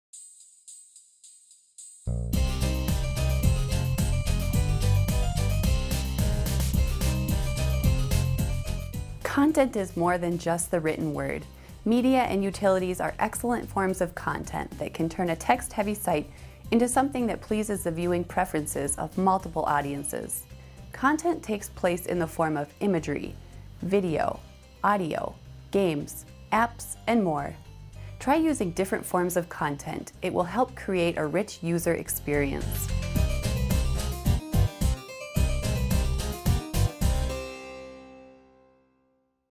Website Must-Haves #19: Other Forms of Content
Media and utilities are excellent forms of content that can turn a text-heavy site into something that pleases the viewing preferences of multiple audiences.